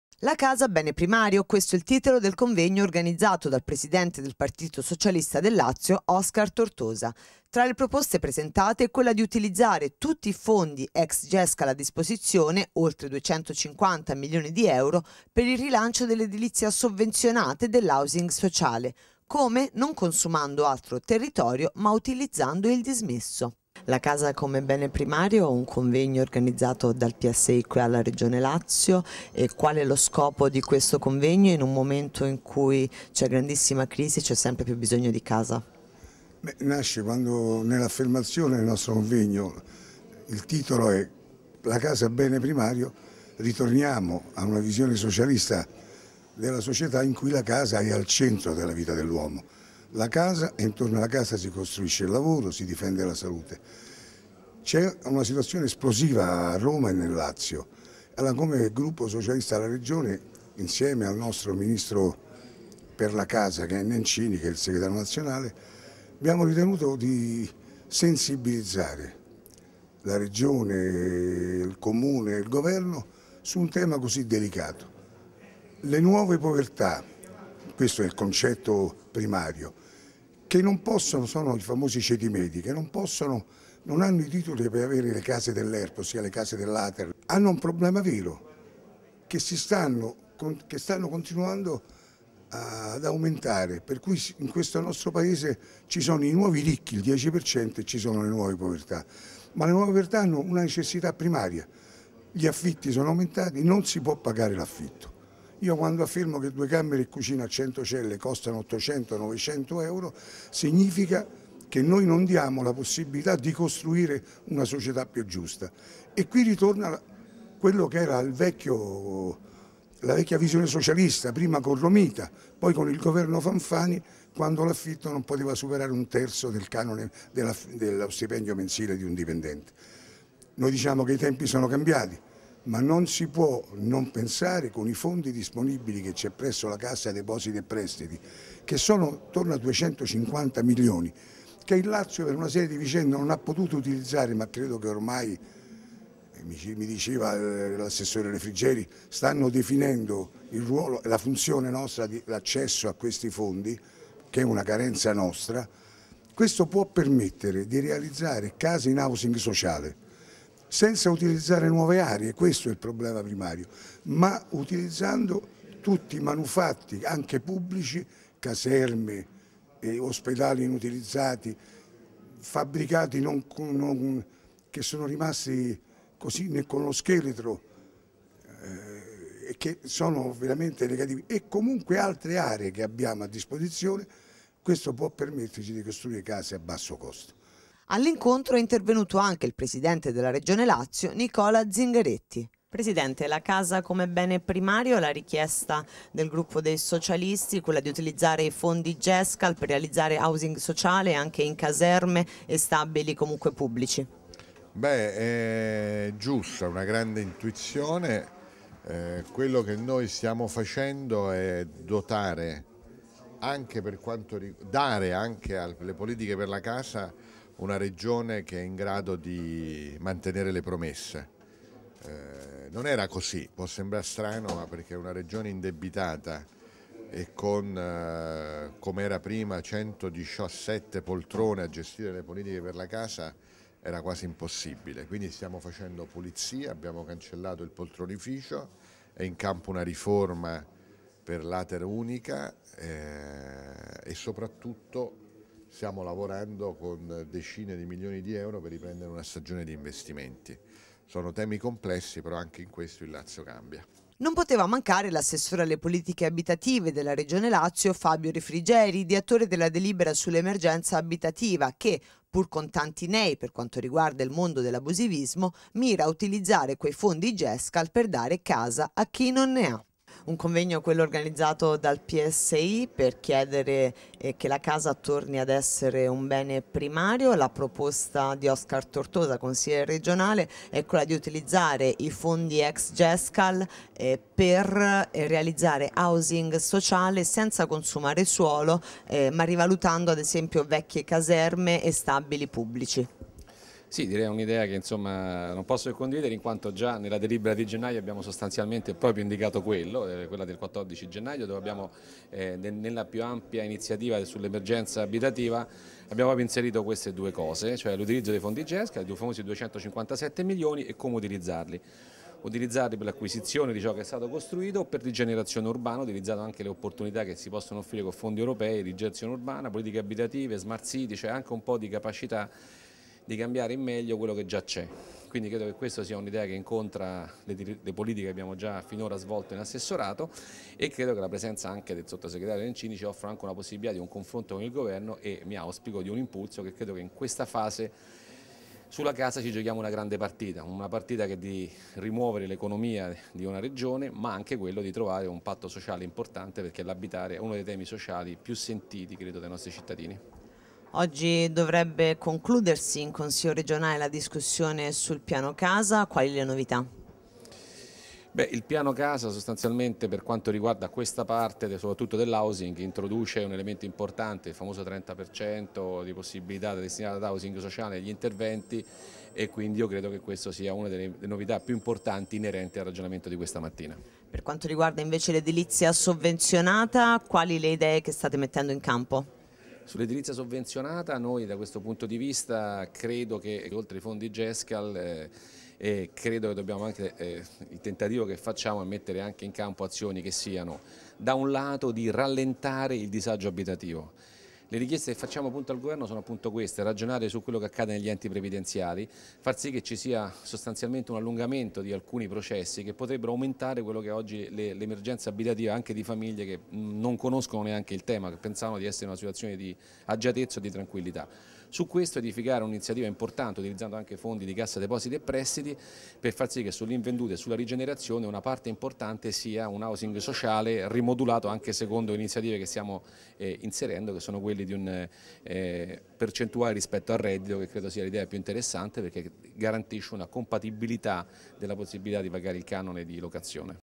La Casa, bene primario
Servizio di Michela Galuppo
Intervista a Tortosa, Zingaretti, Refrigeri